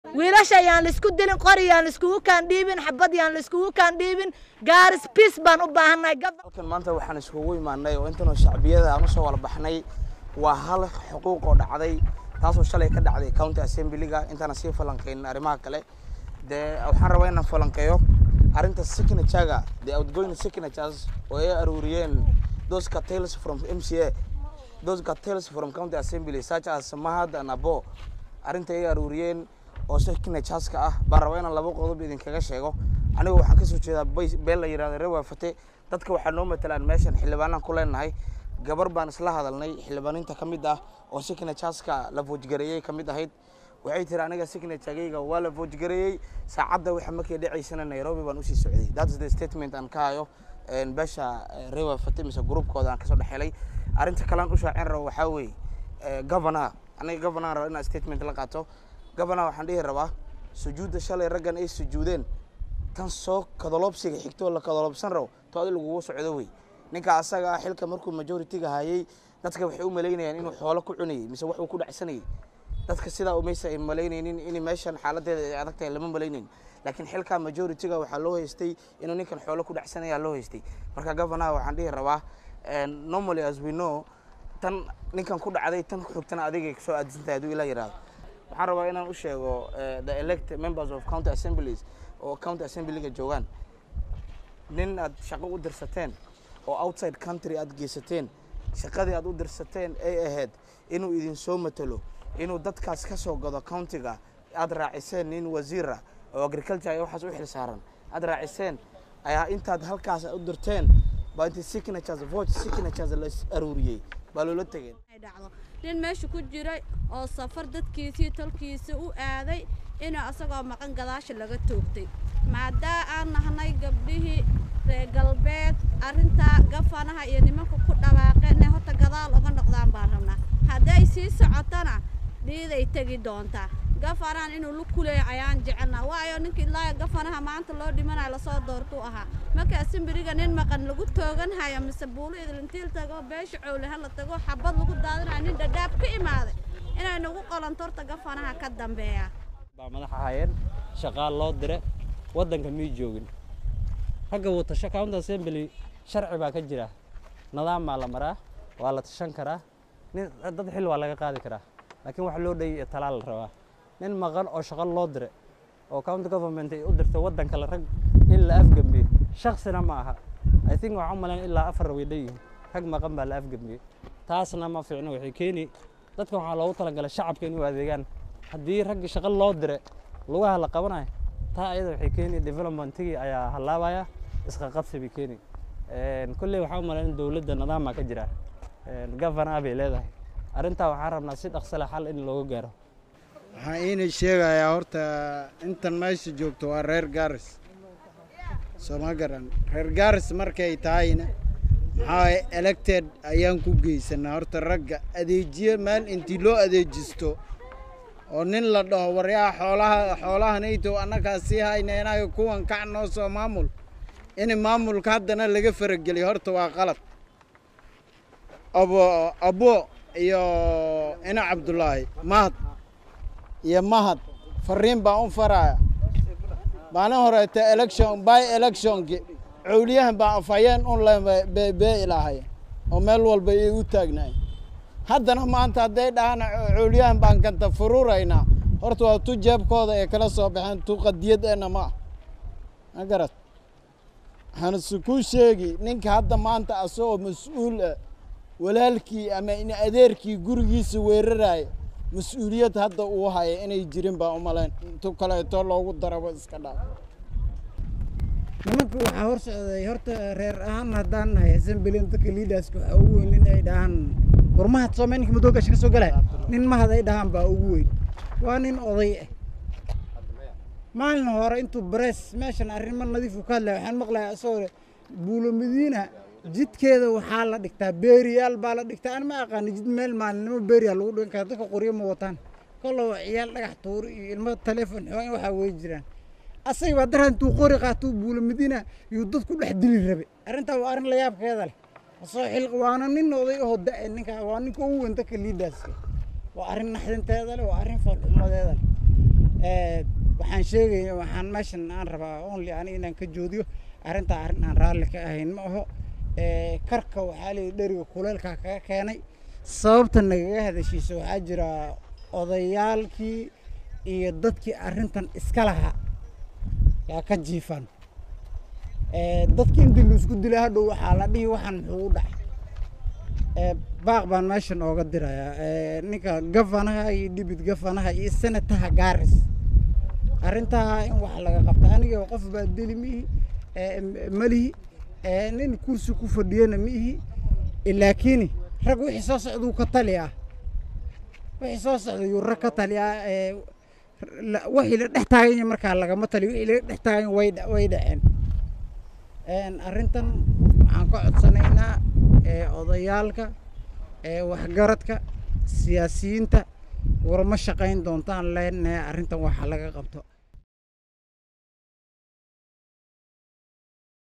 Qaar ka mid ah shacabka countiga Garissa oo walaac ka muujiyay xiisad siyaasadeed oo ka oloolsan...
kulanpost Qaar ka mid ah shacabka countiga Garissa oo walaac ka muujiyay xiisad siyaasadeed oo ka dhalatay xil ka qaadista Hogaamiyaha Aqlabiyadda.